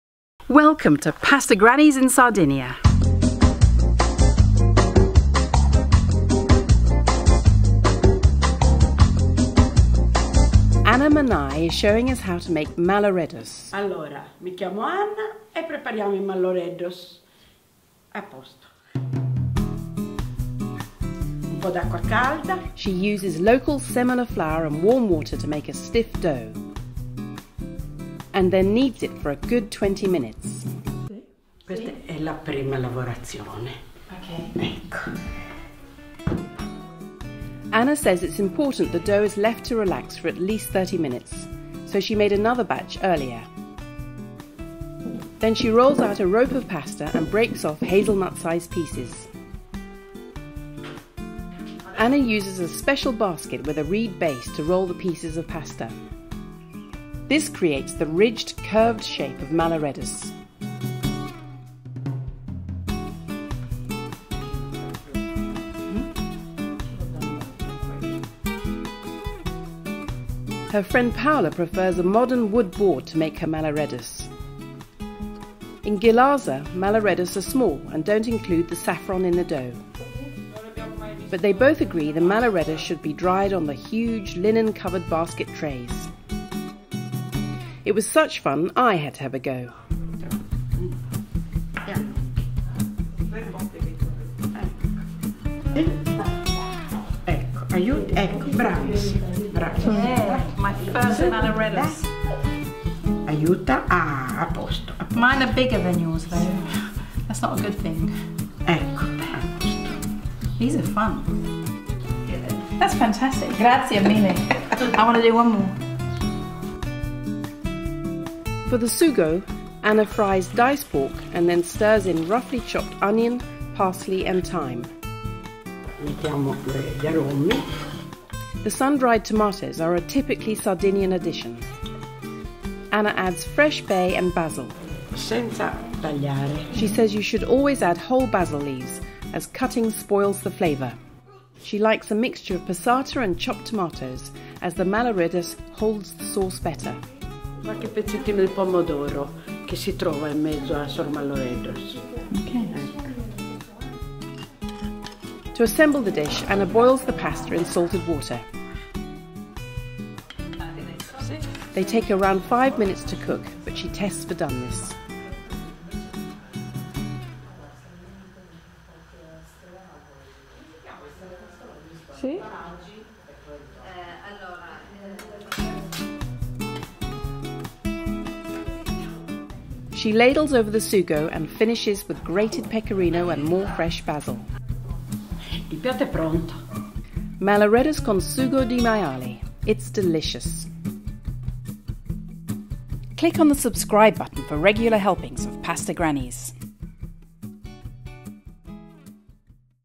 How to Make Malloreddus from Sardinia | Pasta Grannies
We spent a fun afternoon deep in central Sardinia watching Anna Manai make malloreddus with several enormous baskets.  It is a pasta typical of Sardinia with name meaning 'bull calves'  in local dialect. Anna showed us how she likes to serve it with a pork and tomato sauce. Join us for a delicious episode!

Come back next Thursday for more adventures from our trip to Sardinia!

What would you like to see cooked up on the Pasta Grannies channel?